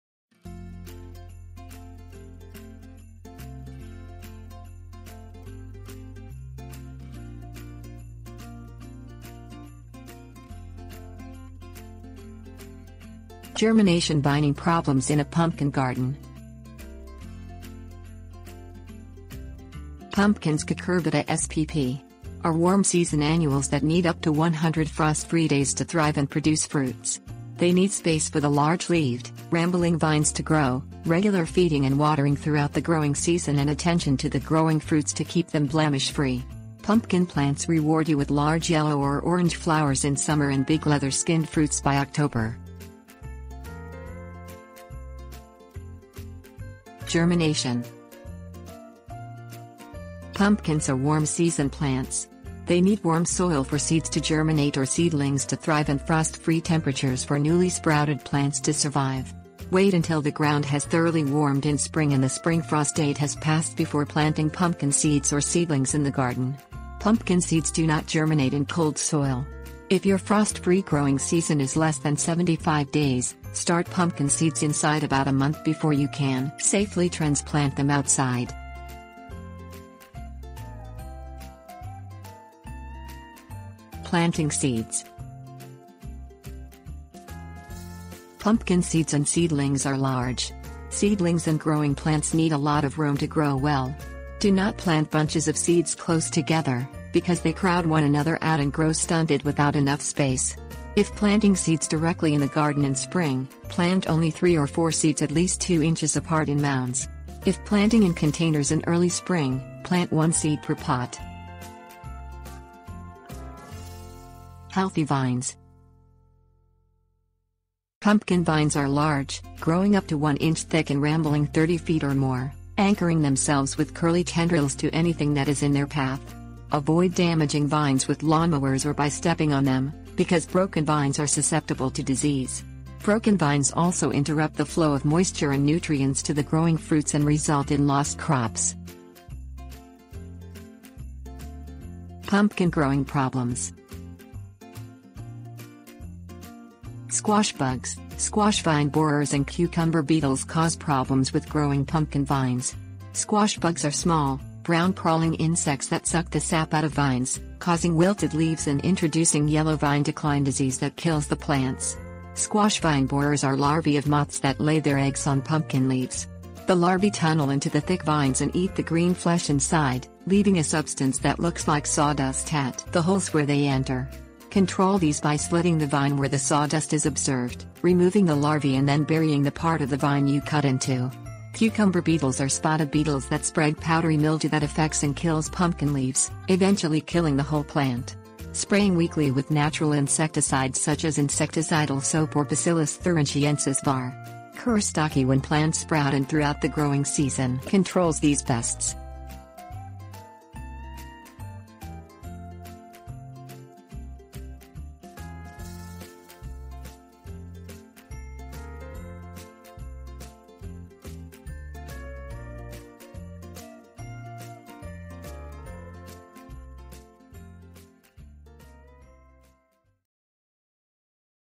Germination & Vining Problems in a Pumpkin Garden
Germination & Vining Problems in a Pumpkin Garden. Pumpkins (Cucurbita spp.) are warm-season annuals that need up to 100 frost-free days to thrive and produce fruits. They need space for the large-leaved, rambling vines to grow, regular feeding and watering throughout the growing season and attention to the growing fruits to keep them blemish free.

Table of contents Germination & Vining Problems in a Pumpkin Garden
Germination 00:45
Planting Seeds 01:22
Healthy Vines 01:53
Pumpkin Growing Problems 02:21